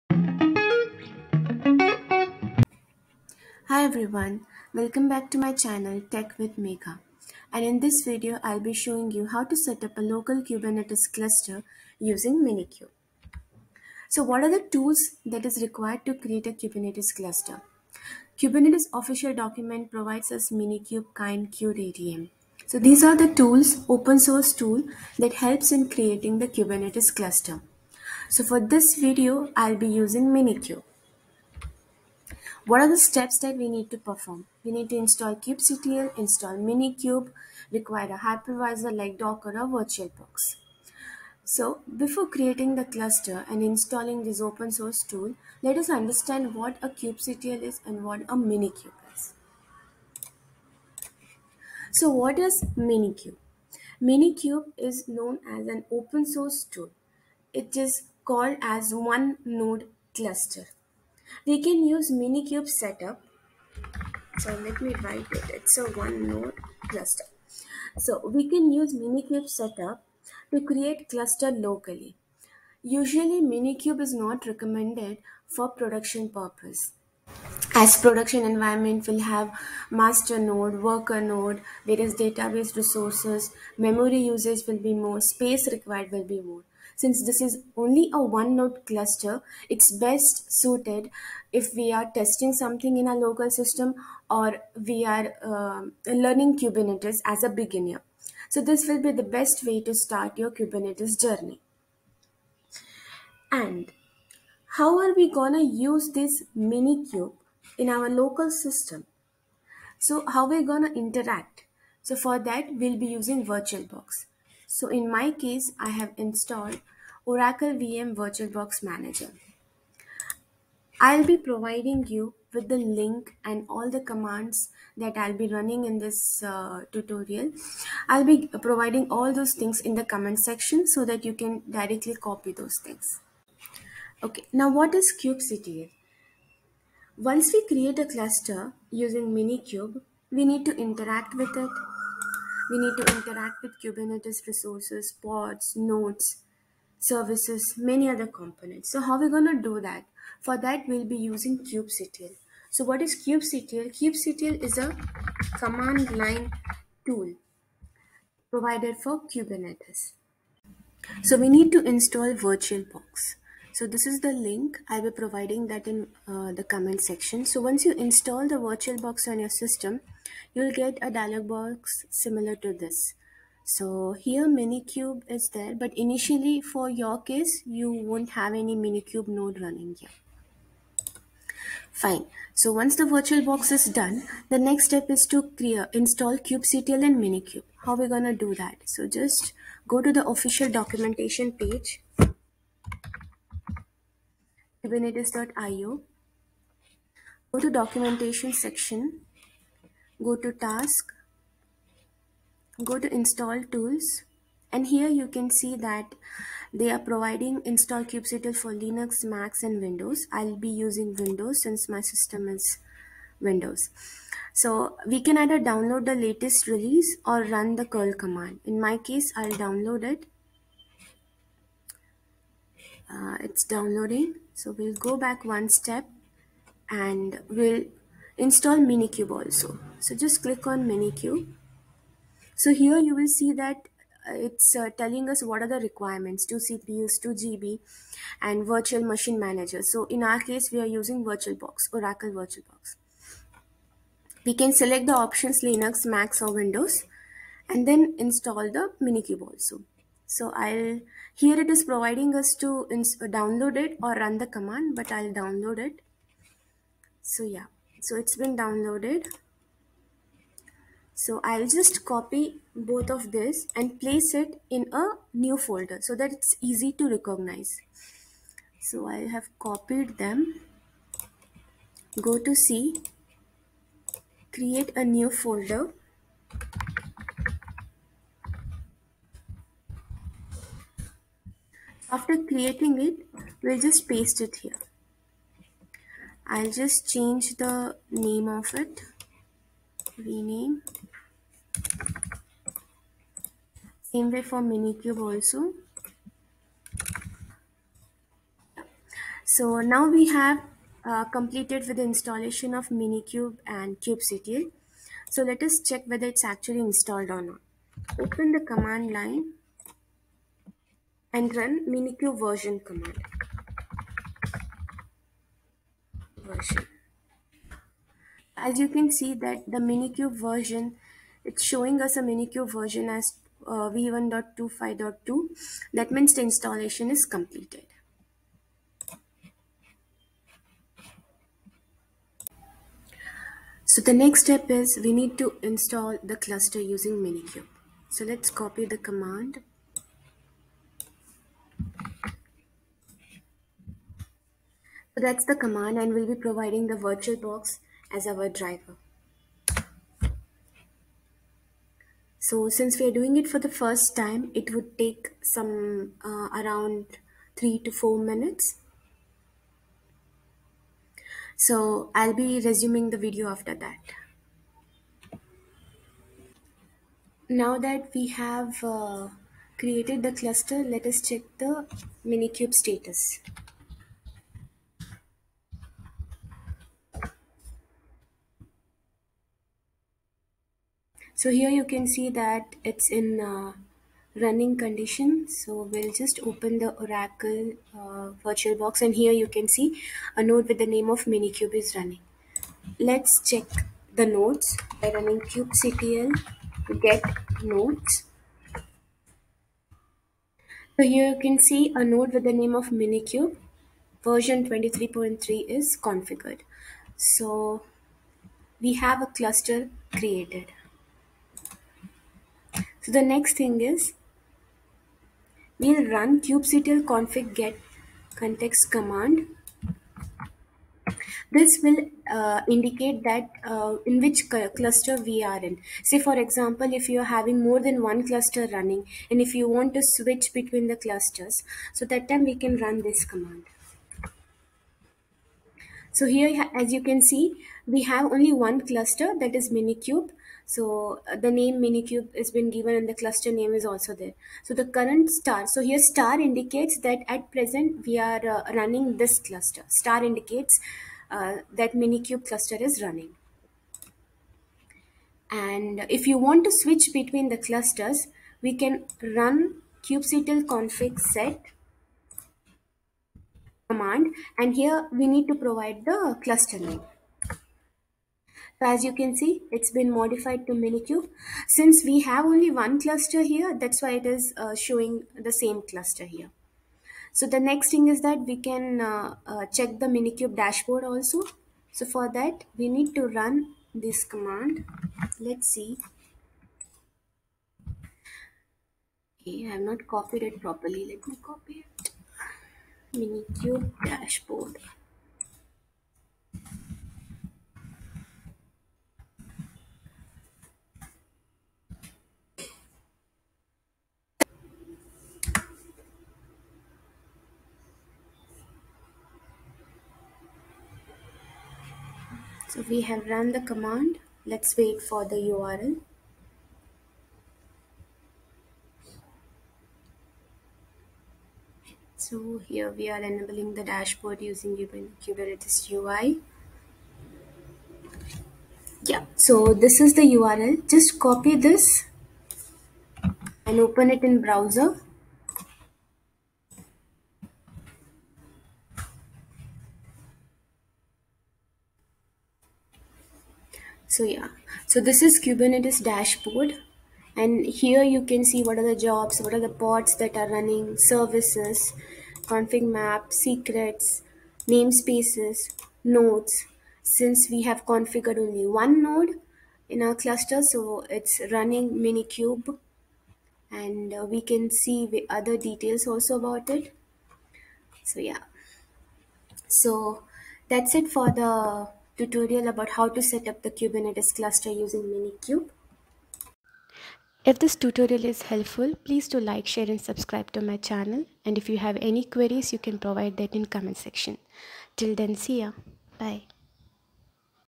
How to create Kubernetes Cluster using Minikube | Install Kubernetes cluster in just 10 mins
"Step 01: Install VirtualBox in your system(Laptop)